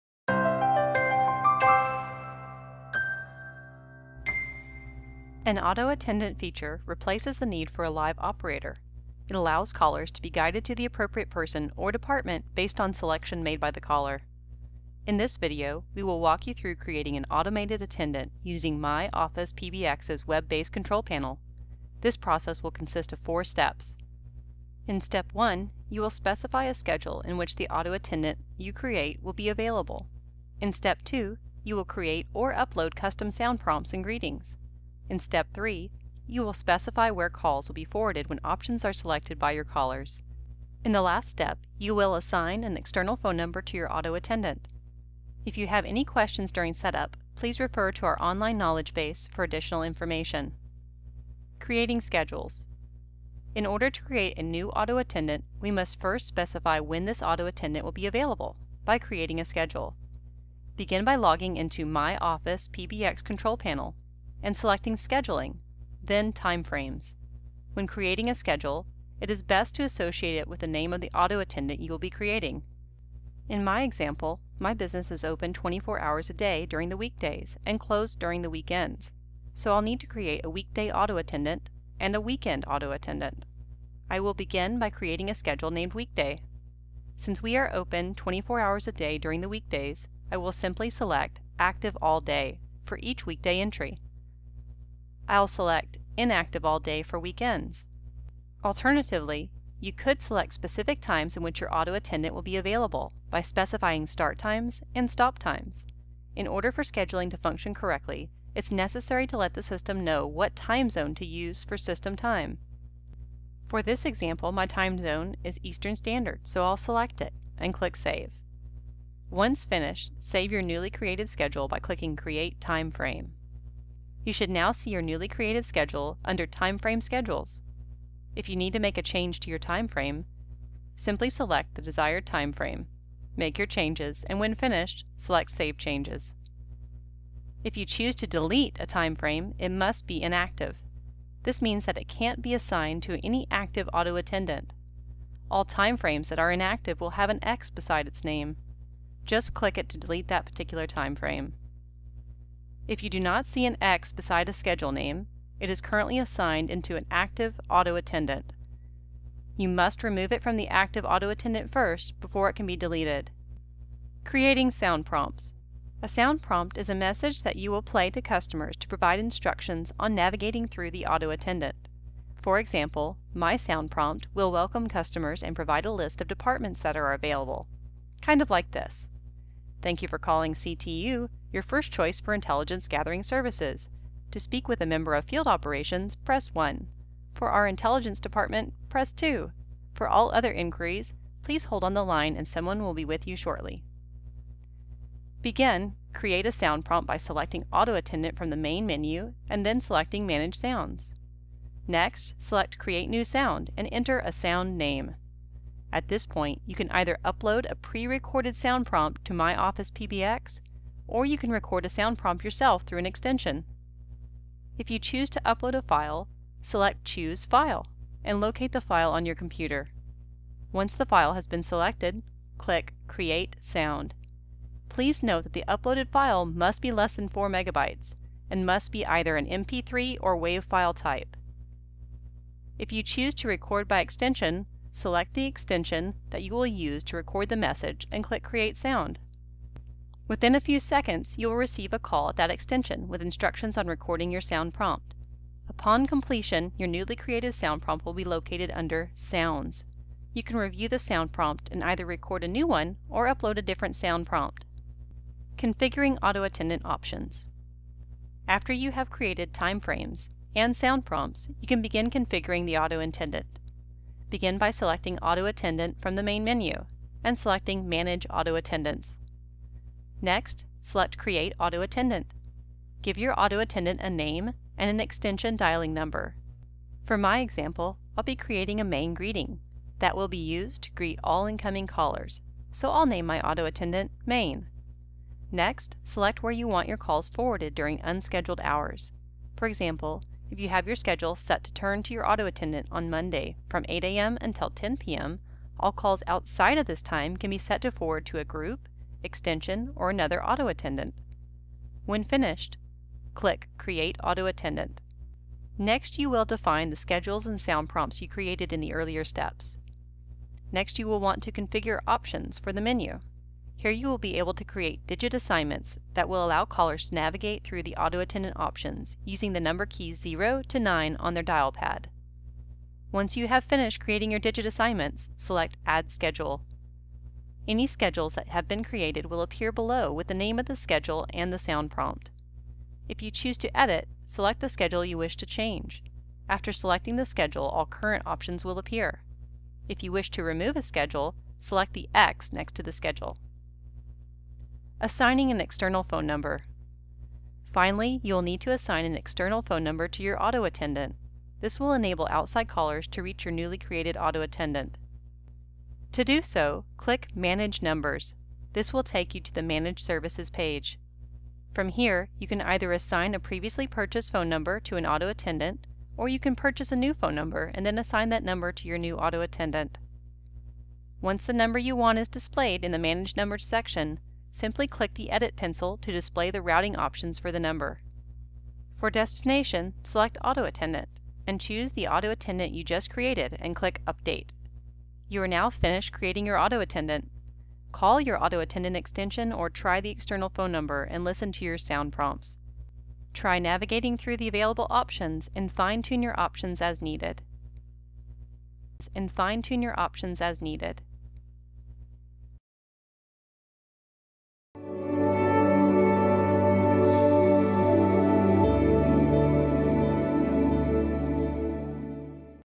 MyOffice PBX: Auto Attendant Configuration
Step by Step instructions on how to create an Auto Attendant with MyOfficePBX

For more details on Auto Attendant Configuration and other tutorials,